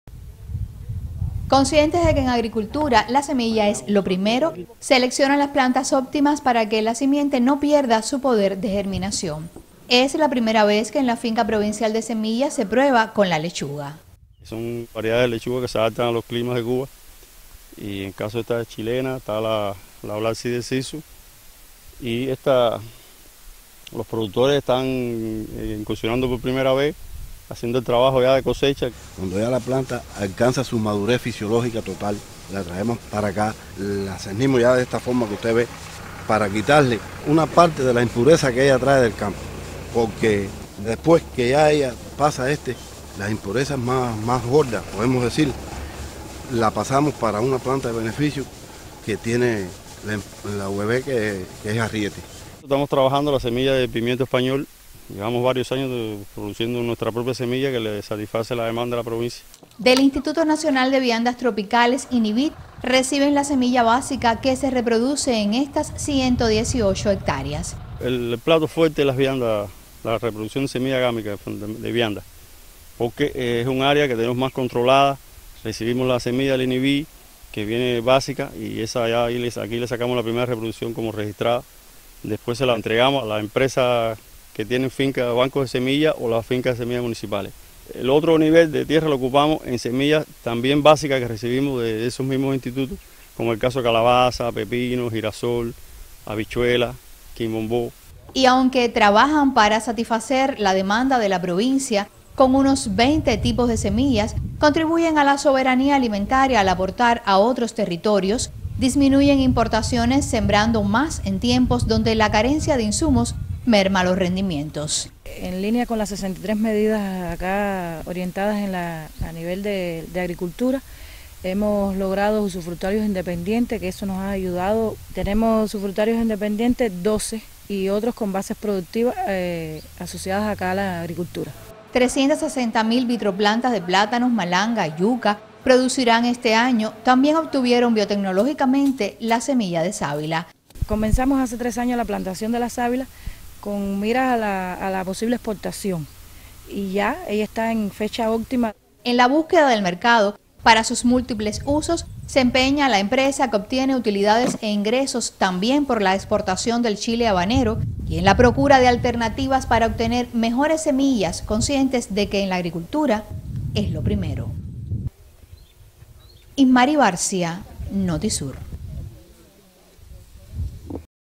Producen semillas en Cienfuegos para garantizar soberanía alimentaria
Producir simientes de calidad y seguras es el propósito de la Empresa Provincial de Semillas en Cienfuegos que prueba variedades más resistentes al clima y extiende sus áreas para paliar así el déficit en los rendimientos, causado por la falta de insumos.